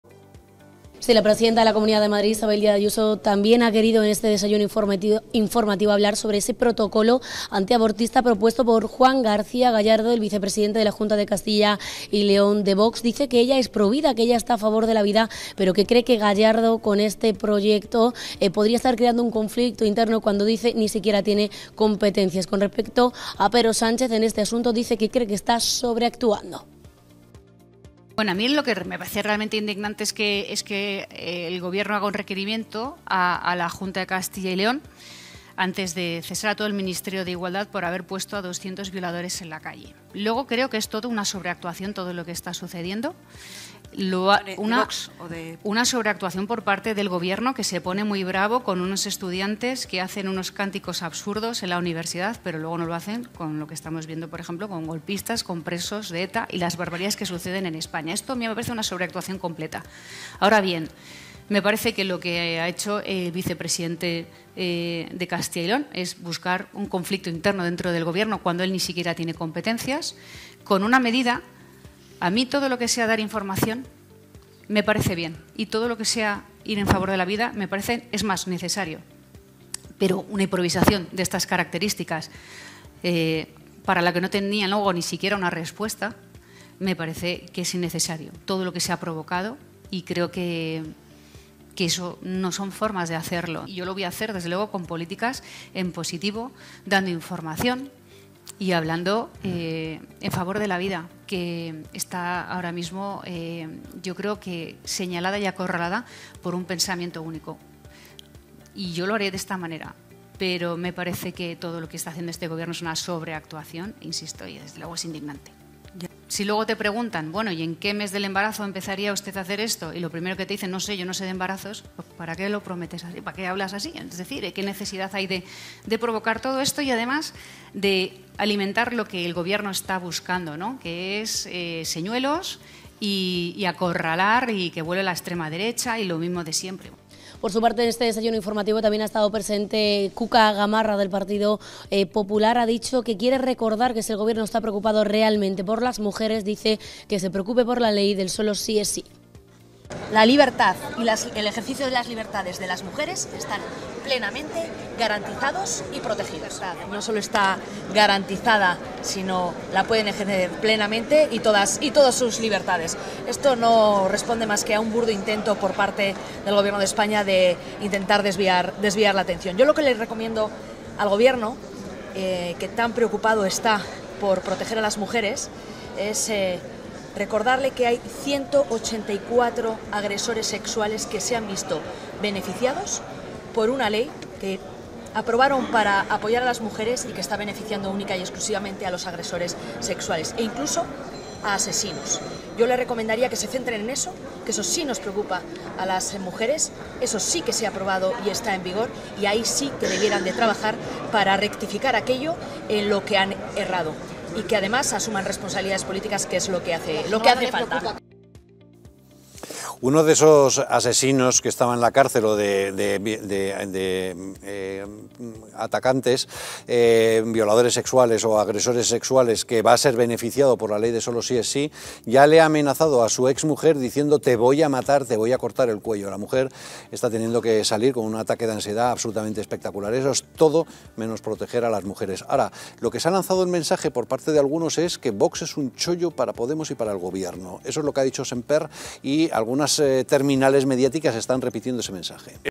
Ayuso considera una 'improvisación' y una 'ocurrencia' la propuesta de Gallardo
Ayuso considera una "improvisación" y una "ocurrencia" la propuesta de Gallardo. Nos lo cuenta Rocío Hernández.

Nuestra Web: 7nn.tv